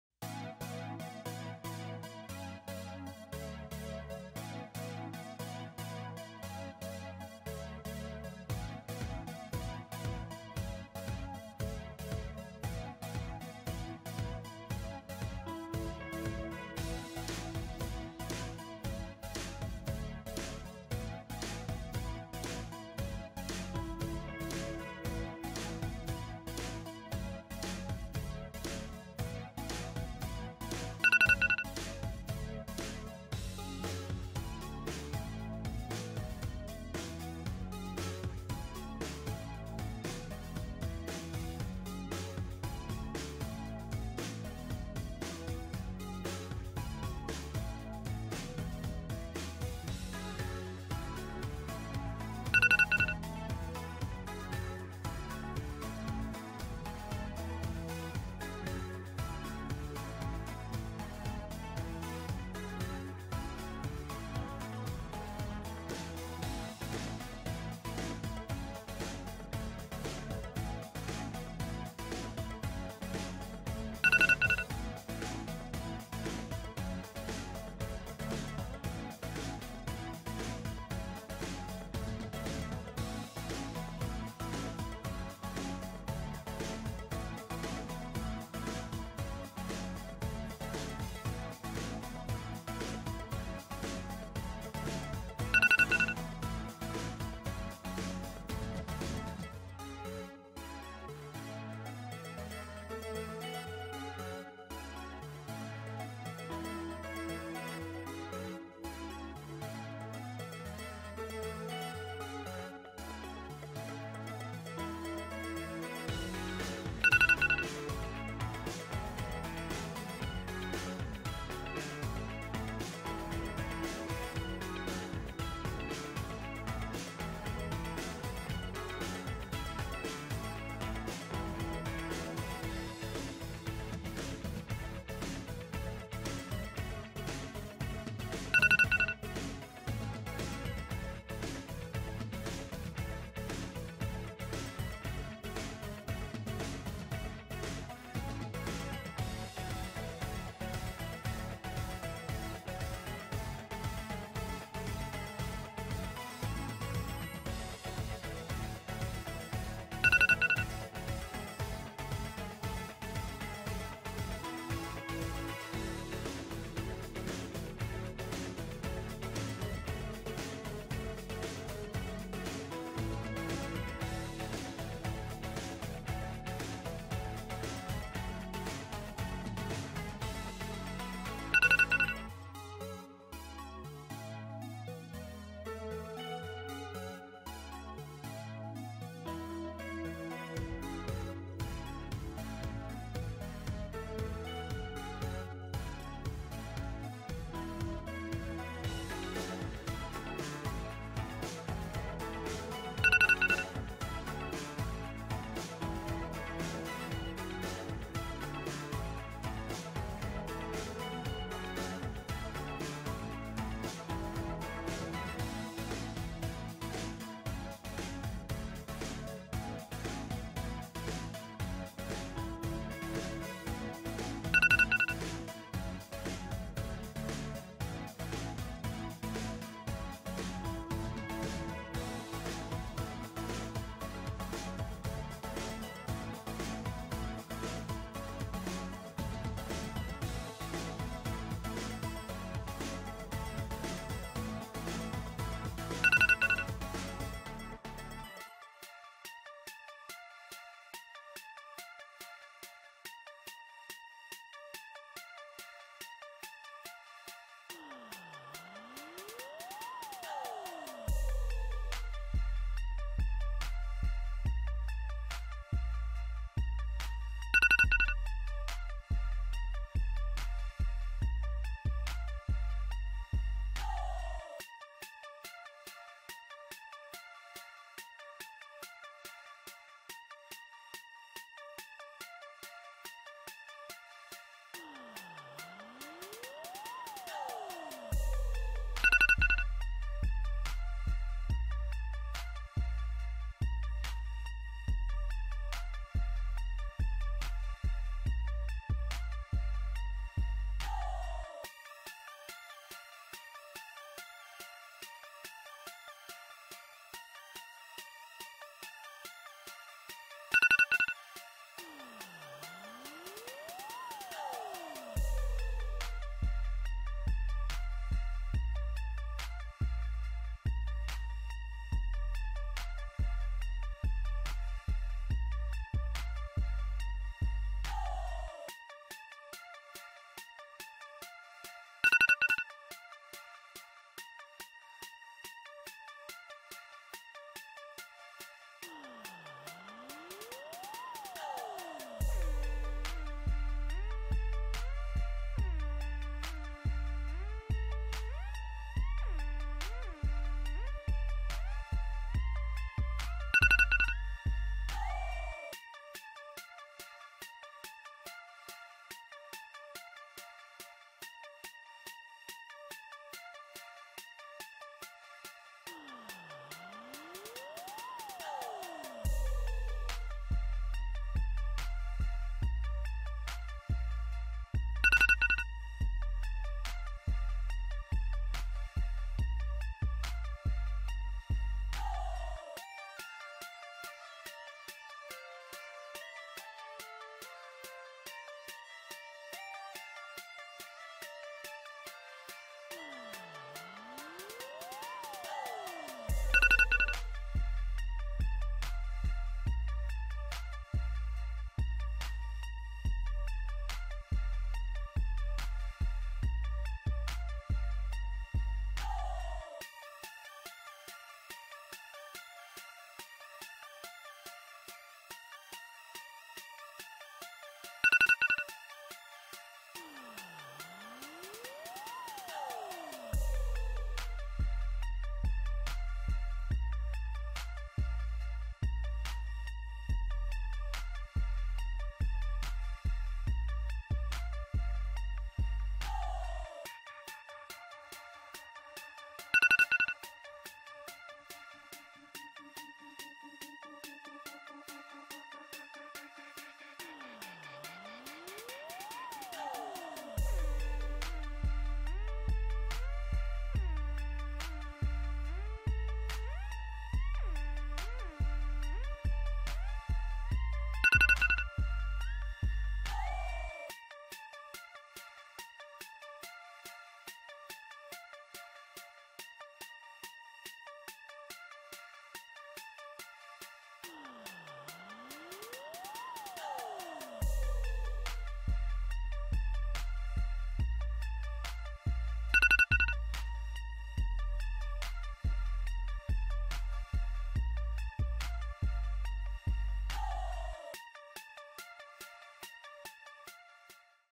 GUESS THE GAME BY THE ITEMS | Videogame Quiz
Try to guess the games by the items, and test your video game knowledge.
Challenge a friend to this quiz too. Have fun